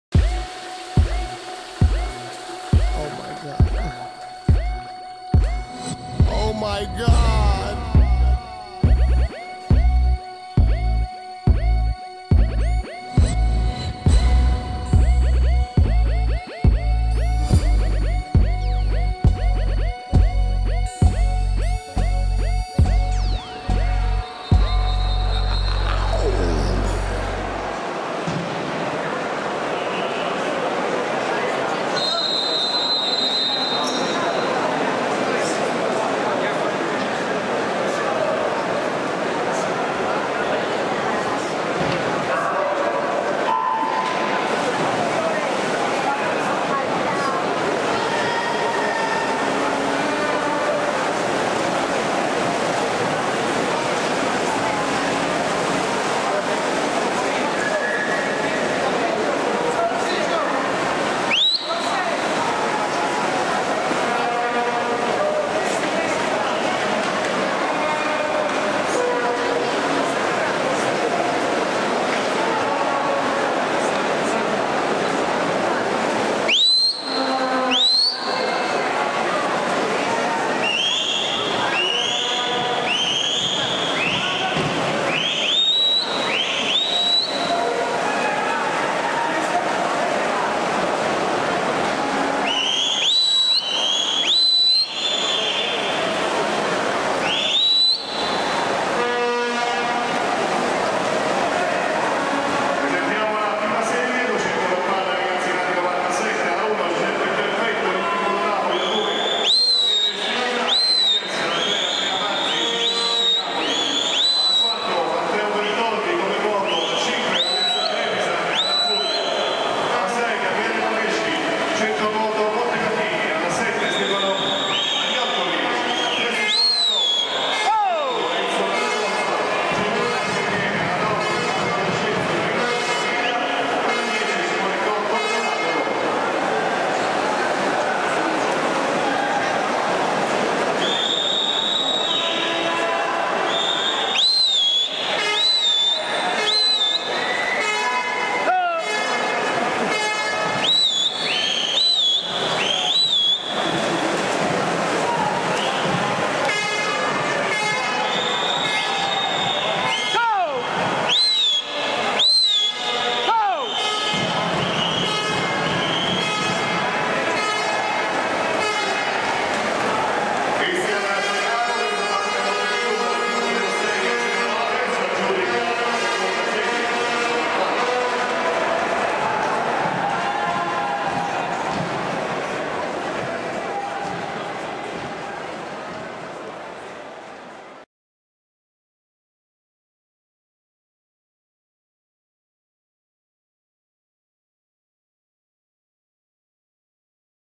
200 FARFALLA MASCHI RAG.97 - CAMPIONATI ITALIANI INVERNALI GIOVANILI - RICCIONE 2012
CAMPIONATI ITALIANI INVERNALI GIOVANILI - RICCIONE 2012
200 FARFALLA MASCHI RAG.97
CON
FERRARO Christian 1997 Veneto Banca Montebelluna 28.65 1'01.00 1'33.29 2'06.09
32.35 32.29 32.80
CALAMI Lorenzo 1997 Rari Nantes Florentia 28.87 1'02.38 1'34.94 2'08.83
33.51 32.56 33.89
TANDA Alessandro 1997 Sport Full Time - Sassari 29.12 1'02.45 1'36.36 2'09.31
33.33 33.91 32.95
MELANI Simone 1997 Centro Nuoto Montecatini 28.30 1'01.37 1'35.47 2'09.33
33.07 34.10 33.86
CARINI Giacomo 1997 Canottieri Nino Bixio 29.12 1'01.90 1'36.72 2'09.34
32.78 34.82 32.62
ALBESIANO Luca 1997 Amatori Nuoto Savona 29.44 1'01.79 1'35.23 2'09.34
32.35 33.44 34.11
NOVARA Filippo 1997 Centro Nuoto Torino 29.03 1'01.77 1'35.28 2'09.38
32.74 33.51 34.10
DI BONA Vincenzo 1997 CN Stelle Marine - Roma 30.00 1'03.35 1'37.36 2'11.96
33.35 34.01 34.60
DI PRIMIO Stefano 1997 Corona d'Abruzzo - Chieti 29.44 1'02.85 1'37.16 2'12.30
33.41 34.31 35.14
DE CHIARA Manuel 1997 Airon Club Bio-Sport ssd 30.53 1'04.17 1'38.72 2'13.11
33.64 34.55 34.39